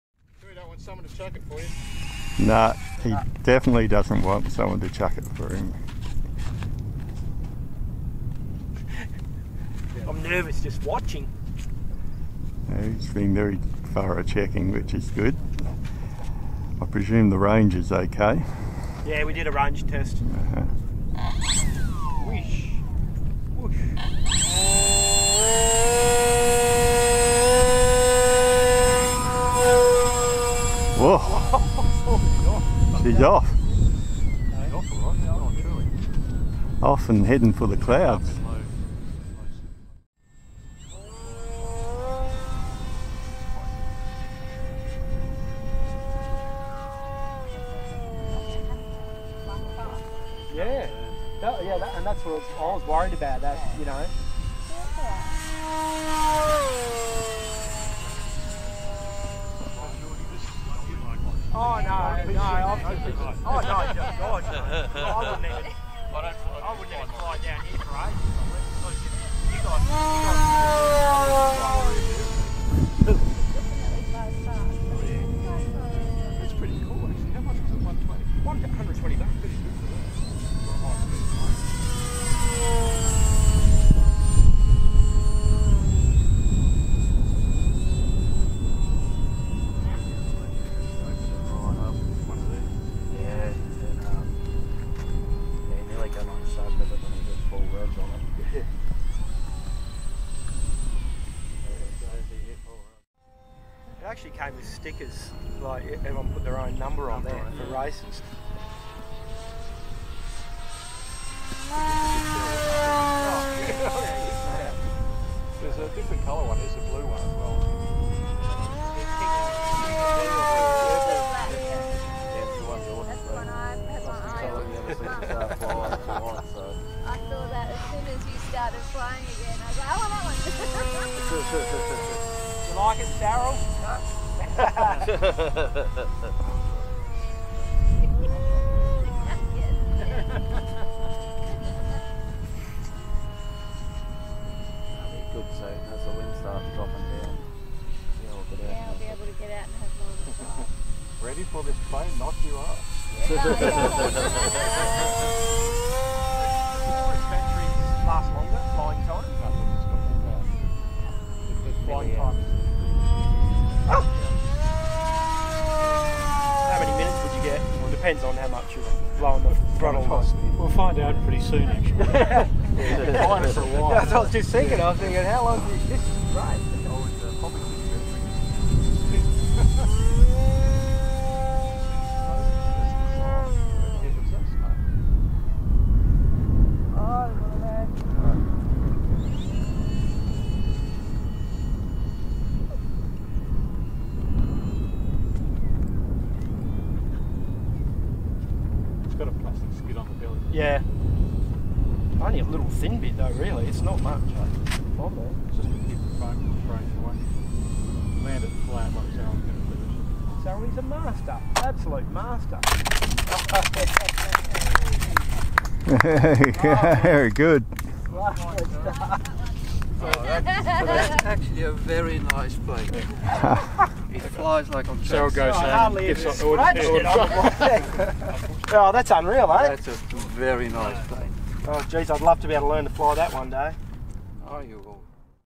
Durafly Goblin Racer maiden flight
Sarel got to fly Ryan's Goblin Racer for the first time and loved it!